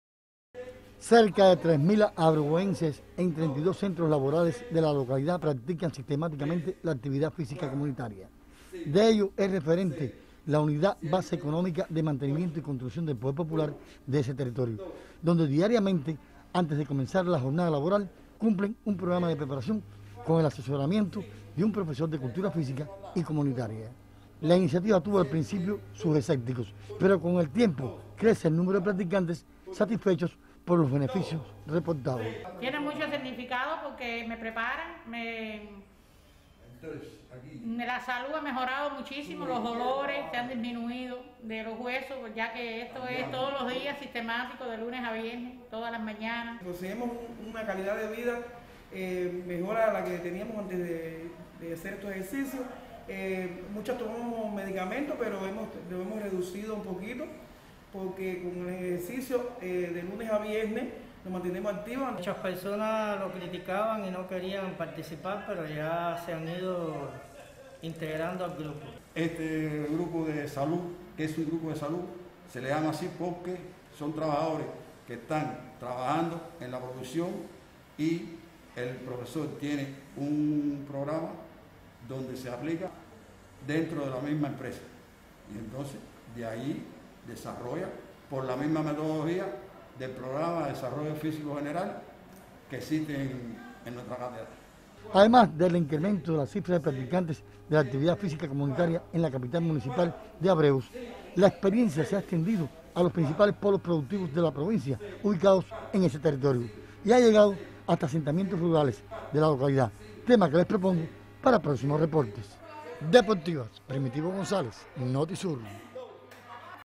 Destaca Abreus por activismo deportivo
El municipio de Abreus, en Cienfuegos se erige  como referente en la práctica de la actividad física laboral con los llamados grupos de salud coordinados por ese sector y el INDER.